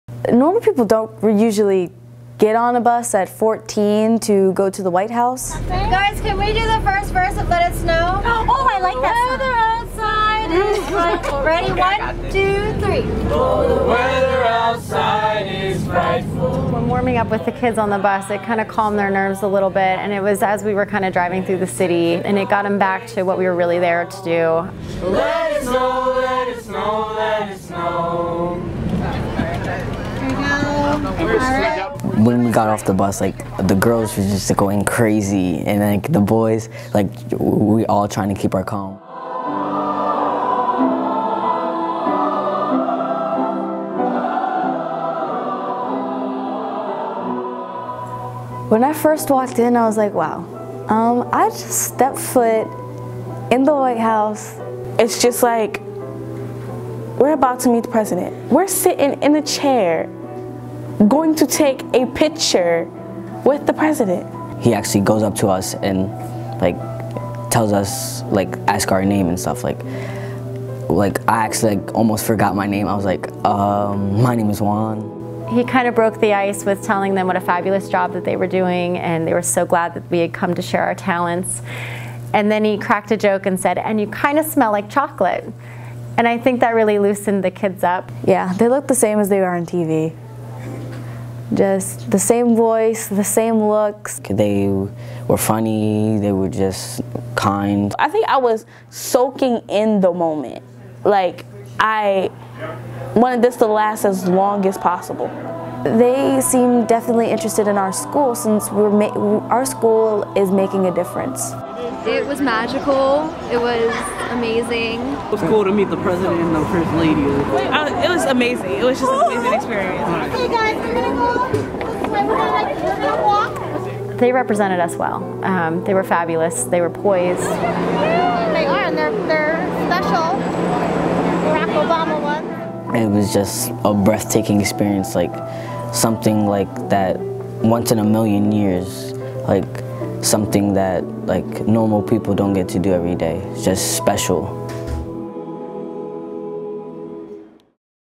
Spartan Singers meet President Obama
On Monday, December 12, 2016, 24 students in the Middle Division Select Choir traveled to Washington D.C. to perform at one of the White House's annual Christmas receptions. The students greeted guests with festive holiday tunes as they entered the White House.

President Obama and the First Lady made an appearance during our students' two-hour performance, adding to the excitement of an already enjoyable day. To top it off, White House social secretary and MHS alumna Deesha Dyer '95 was able to reconnect with her fellow Milts during this celebratory event.

This performance was historic for the Milton Hershey School community as it marks the first time our students have been invited to perform at the White House!